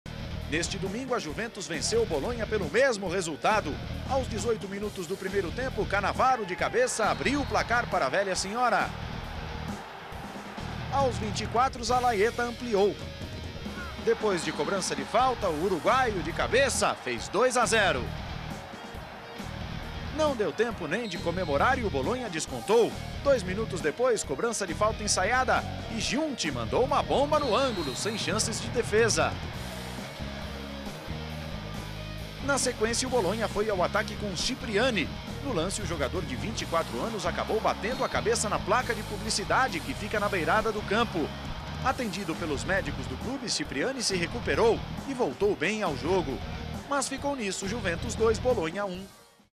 Juventus 2 x 1 Bologna - Campeonato Italiano 2004/2005
Estádio: Delle Alpi - Turim-Itália
Gols:
1° Fabio Cannavaro 18'/1 (Juventus)
2° Marcelo Zalayeta 24'/1 (Juventus)
3° Federico Giunti 24'/1 (Bologna)